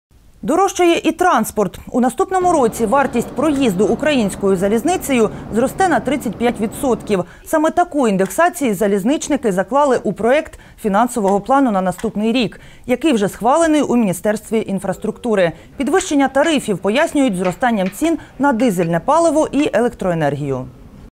Квитки на залізниці здорожчають на 35%
Дорожчає і транспорт. У наступному році вартість проїзду українською залізницею зросте на 35%. Саме таку індексацію залізничники заклали у проект фінансового плану на наступний рік, який вже схвалений в Міністерстві інфраструктури. Підвищення тарифів пояснюють зростанням цін на дизельне паливо і електроенергію. А також на підтримку вагонів та локомотивів.